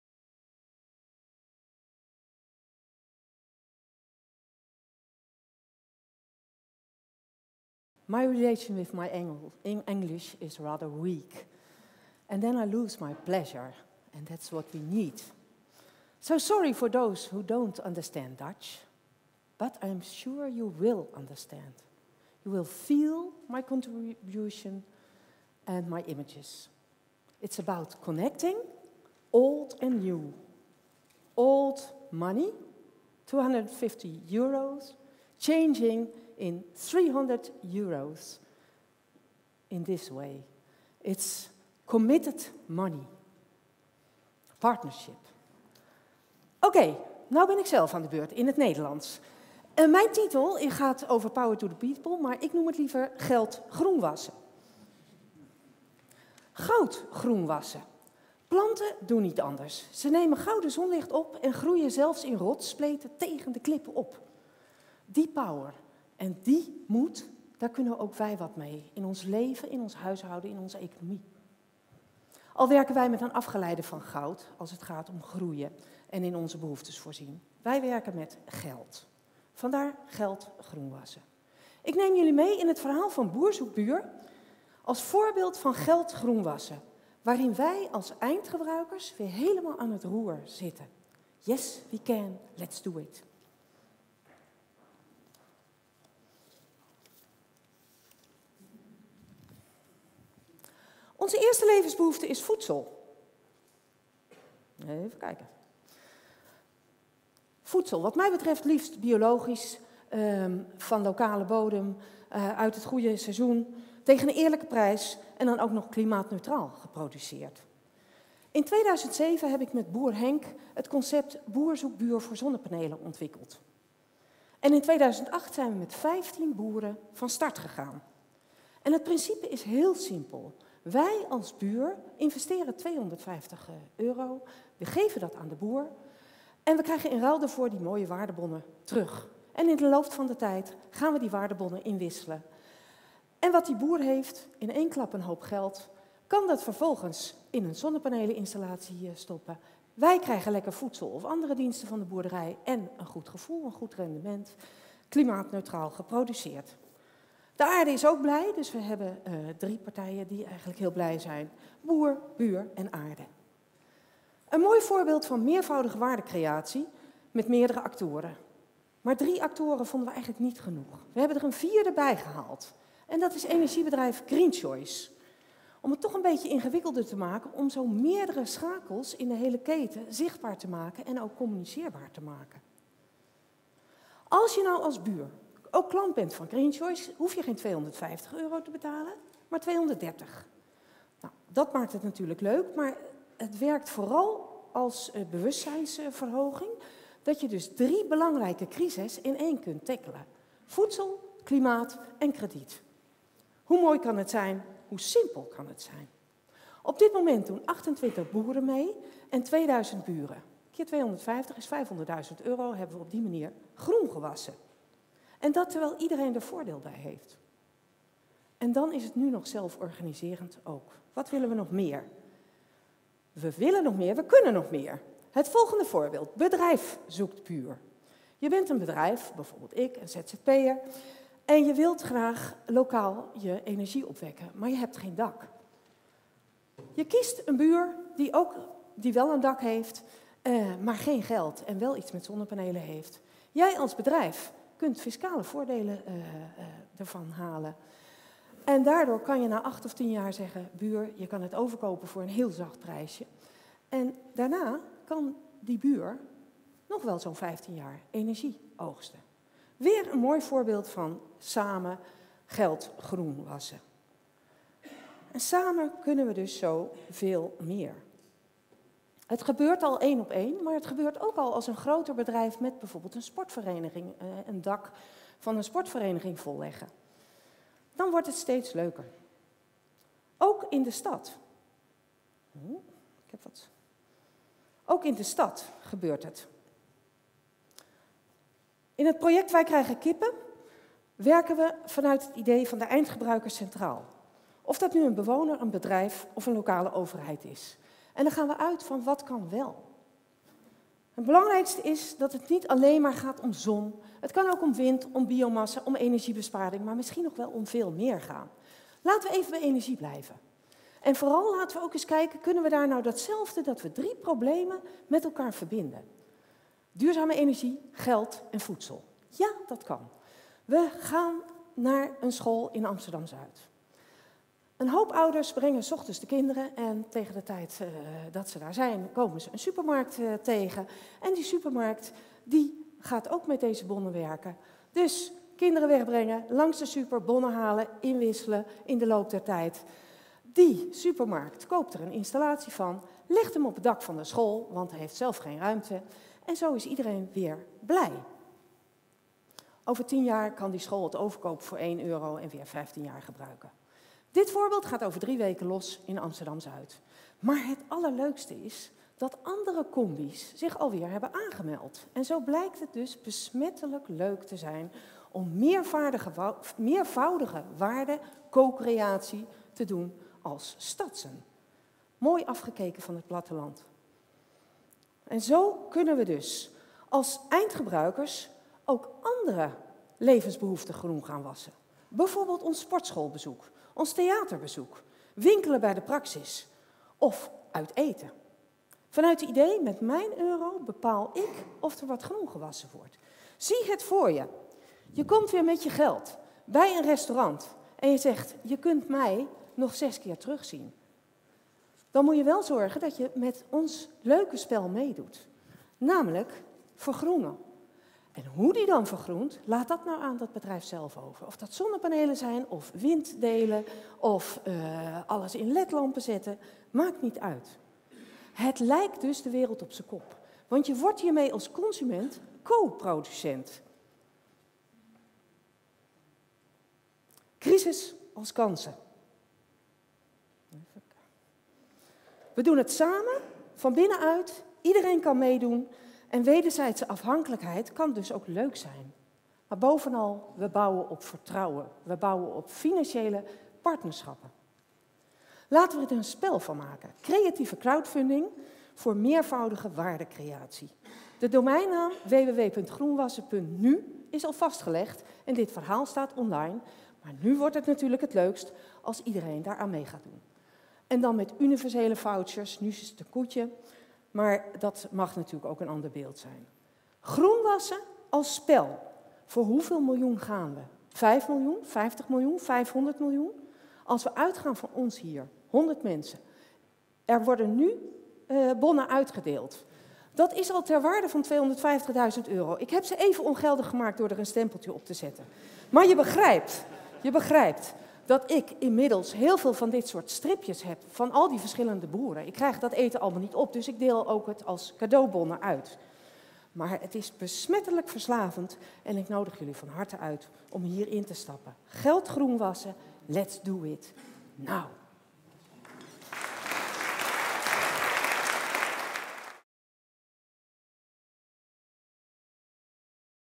Power to the people, about community organized energy: Anne Stijkel at TEDxLeiden
Anne Stijkel tells the story of some innovative practices to organize sustainable energy.